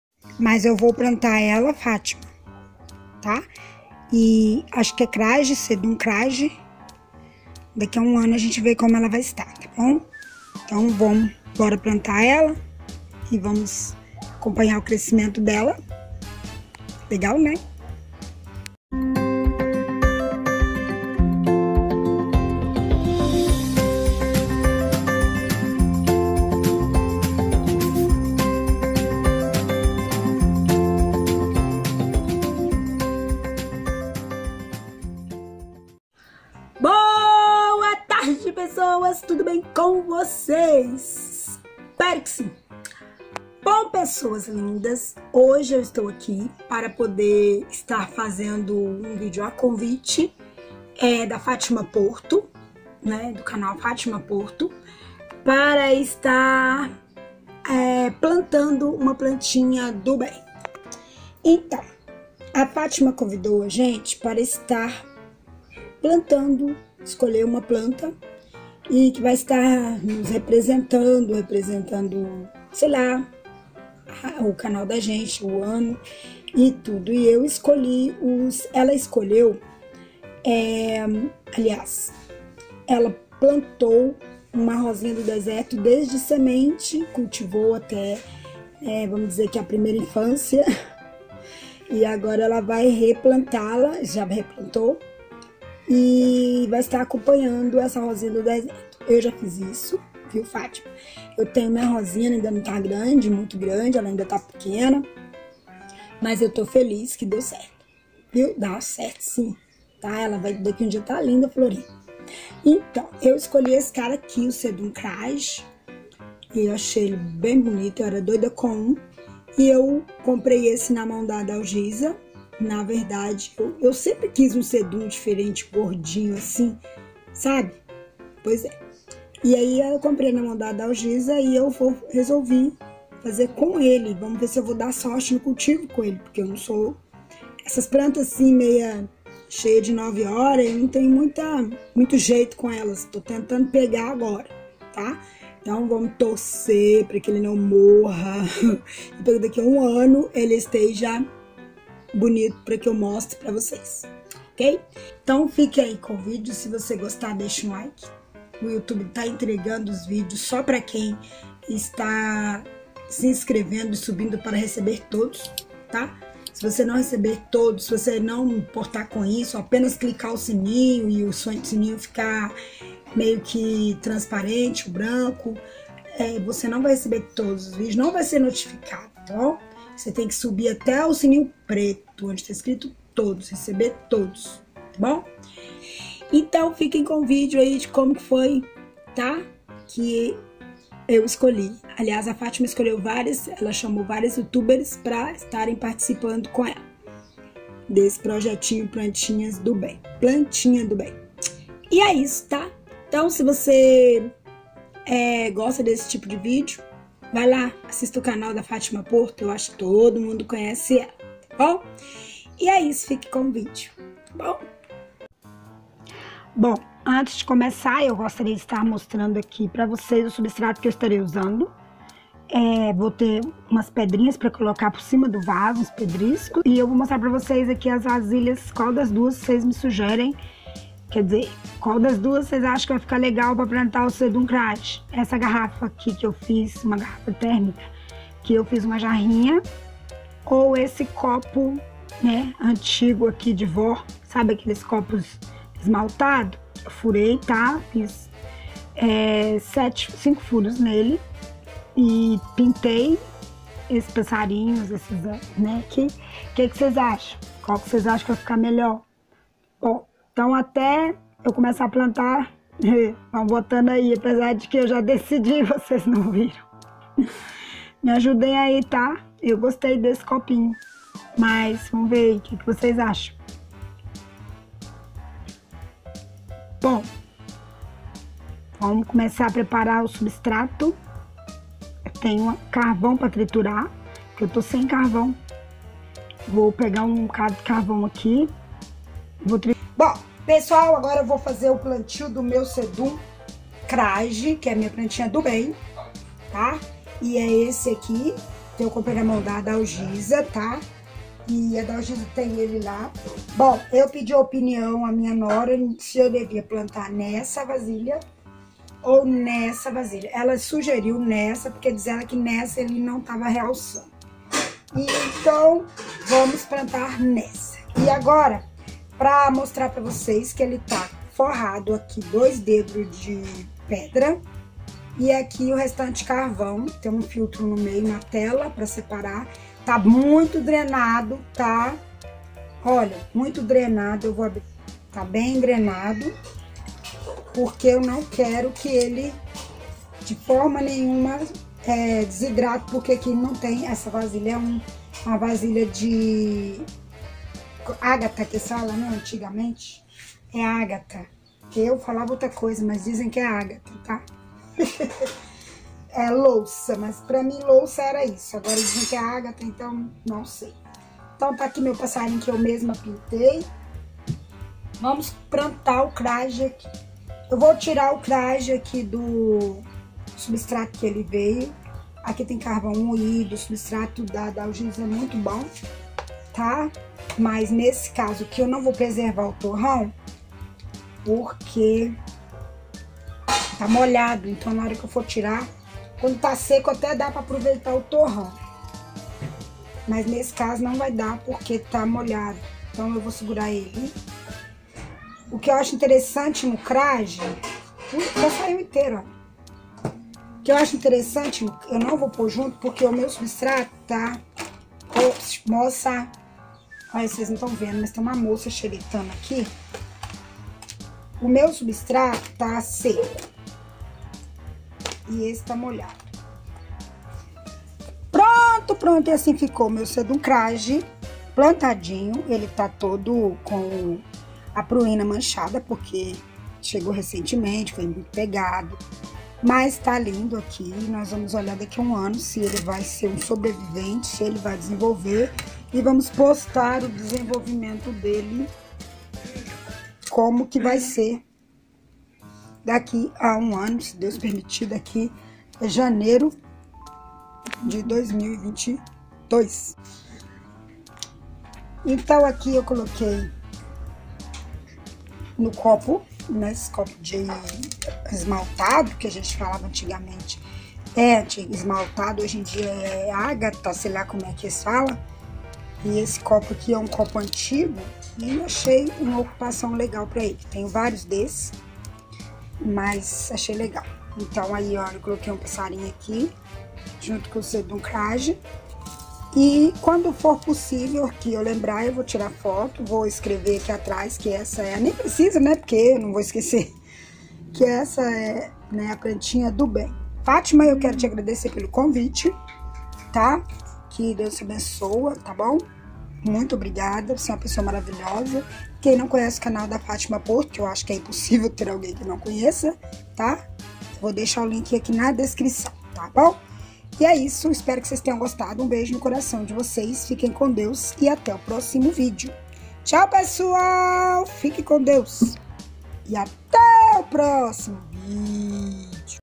PLANTINHADOBEM PROJETOFÁTIMAPORTO SEDUMCRAIGII
Fui convidada para participar do projetoplantinhadobem, criado pela Fátima Porto, para plantar e mostrar uma plantinha agora no mês de janeiro de 2021, e acompanhar o crescimento dela, e tornar a mostrar seu desenvolvimento em janeiro de 2022.Qdo a Fátima tornará a nós convocar pra mostrarmos o qto se desenvolveu a nossa plantinhadobem.